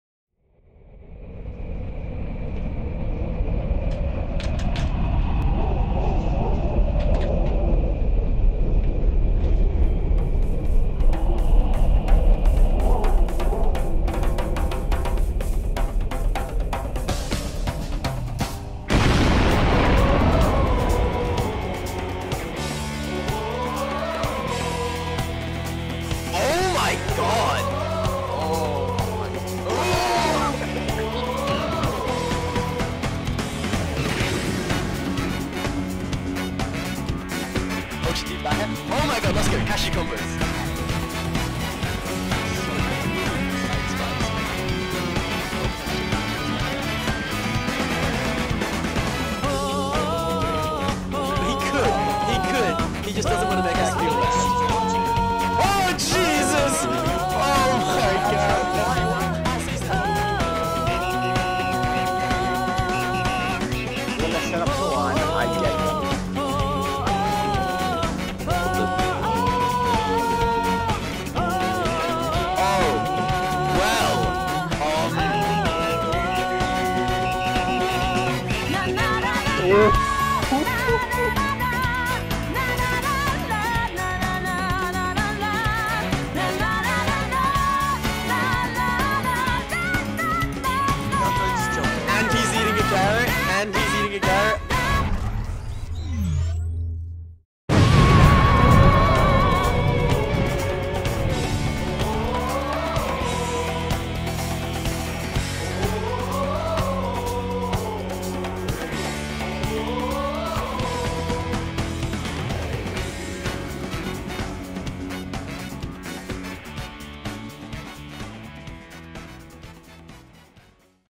CASHEW COMPILATION GRAND FINALE: THE CLASH OF THE COMPILATIONS, OFFICIAL TRAILER!!!
REMASTERED COMPS, REMIXED MUSIC AND NEW LOST CLIPS FOUND!!!
ITS FINALLY COMING! The long awaited and teased finale to the cashew compilation series is coming on Sunday the 30th of May AWST, and a little surprise on the 28th of May as well SO STAY TUNED!!!